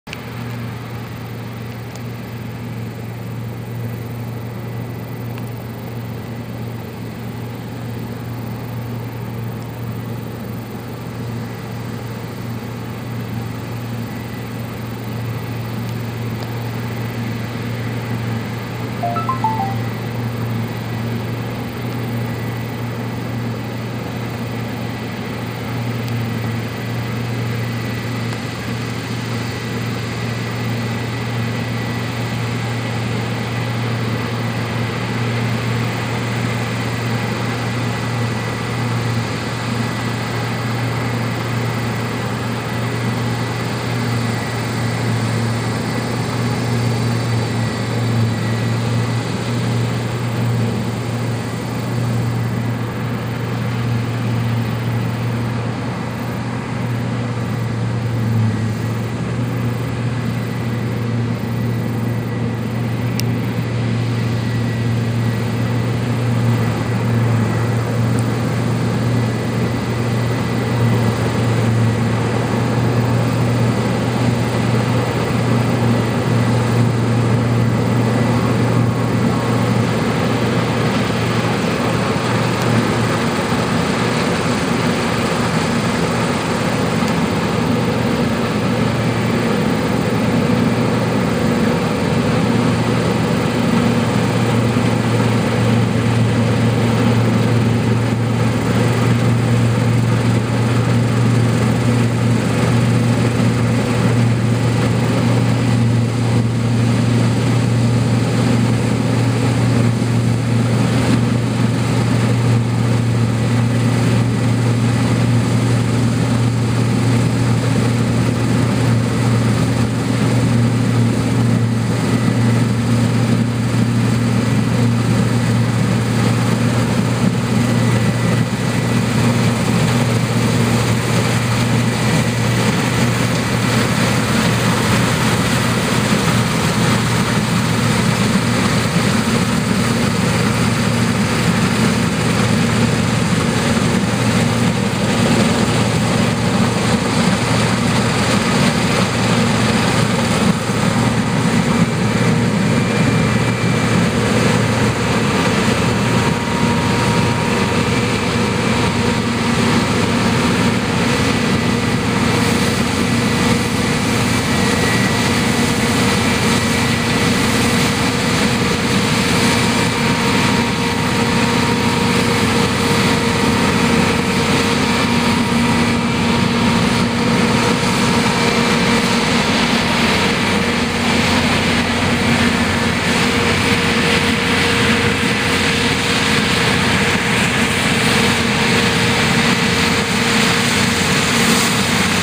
Moisson blé 2017 : New NH CX 8.70. (Partie 1)
Moisson de blé ds la Marne avec une moissonneuse New Holland CX 8.70 au commencement du champ et à la vidange trémie avec un tracteur New Holland T 7050 attelé à une benne Brimont 18T.